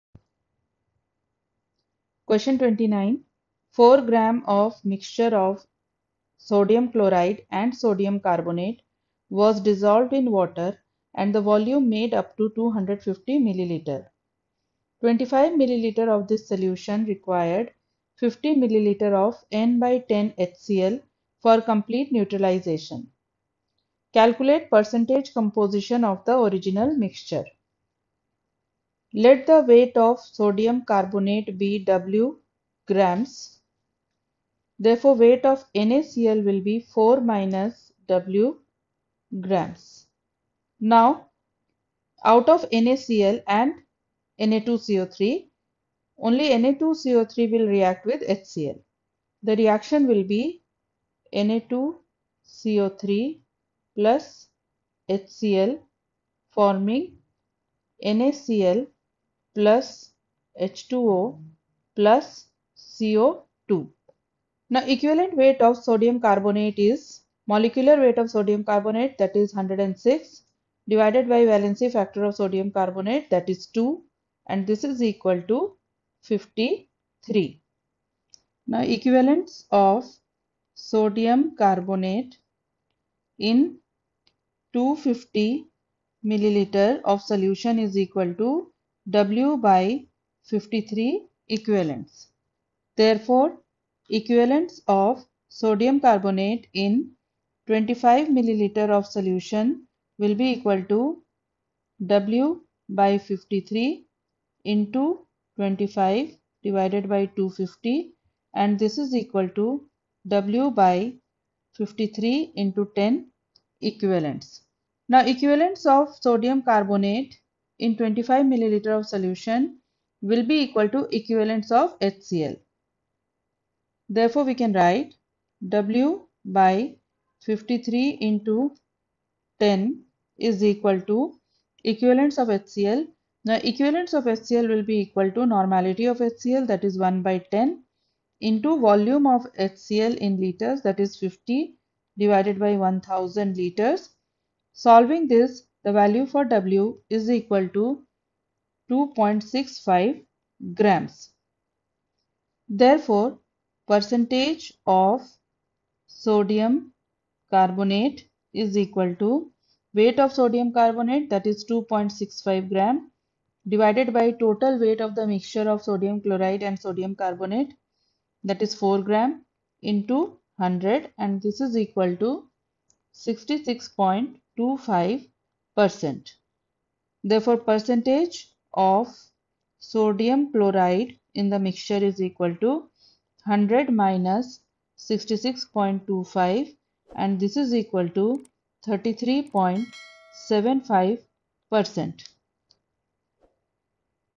Q29. 4.00 g of mixture of NaCl and Na2CO3 was dissolved in water and the volume made up to 250 ml;
Ch7 Q29. 4.00 g of mixture of NaCl and Na2CO3 was dissolved in water and the volume made up to 250 ml; 25, mL of this solution required 50 ml of N/10 HCI for complete neutralisation. Calculate percentage composition of the original mixture.